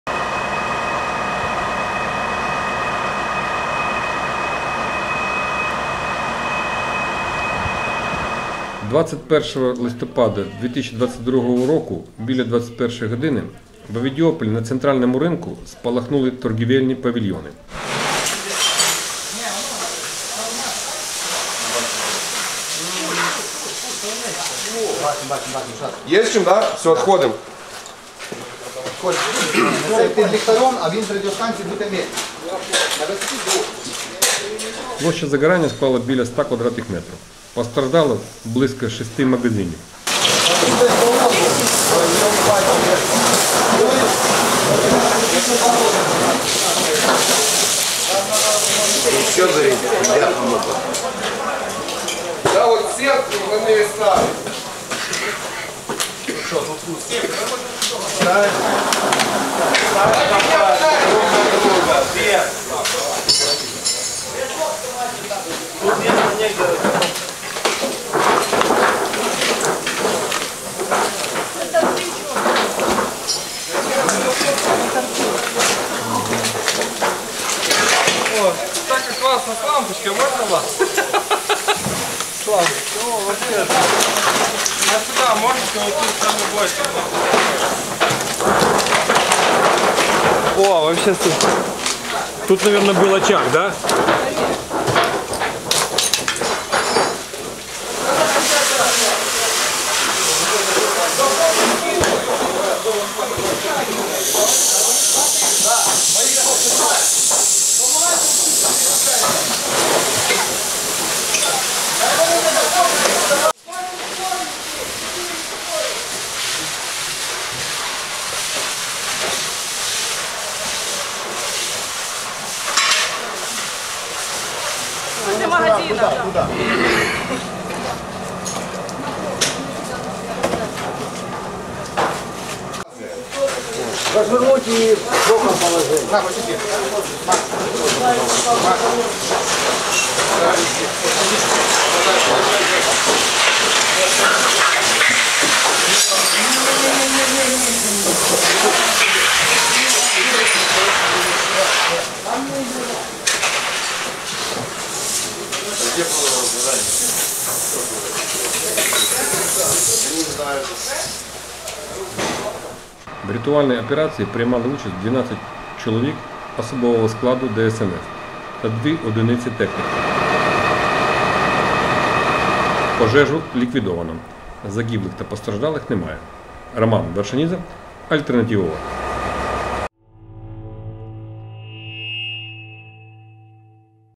В ОВІДІОПОЛІ НА РИНКУ СПАЛАХНУЛИ ПАВІЛЬЙОНИ
21 листопада 2022 року біля 21:00 в Овідіополі на Центральному ринку спалахнули торговельні павільйони.
Площа загорання склала біля 100 м2. Постраждало близько шести магазинів.
В рятувальної операції приймали участь 12 чоловік особового складу ДСНС та 2 одиниці техніки.
Пожежу ліквідовано. Загиблих та постраждалих немає.
Роман Варшанідзе
АЛЬТЕРНАТИВА.ОРГ